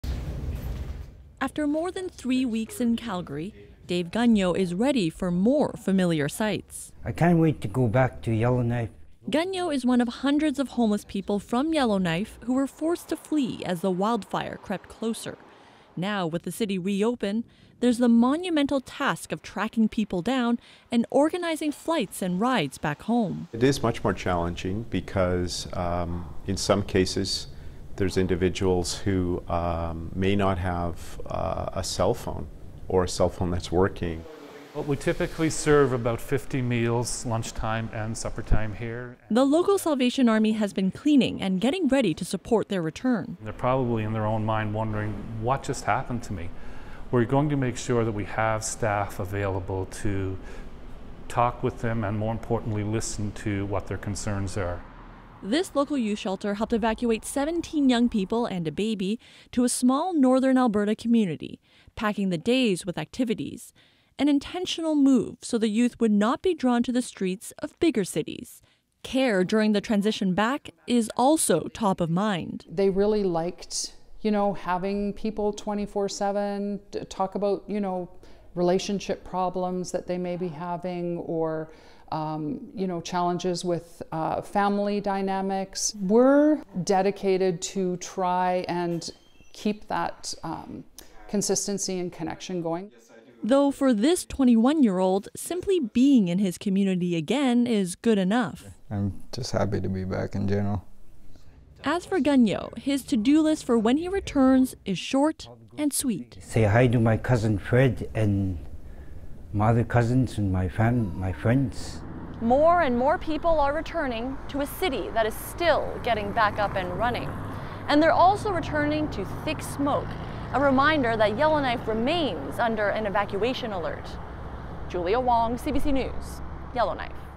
Yellowknife officials move to help homeless residents as city reopens
Weeks after the city was evacuated due to dangerous wildfires, Yellowknife residents are trickling back in, including those without homes of their own. Local agencies are now preparing supports, including meals and temporary shelter.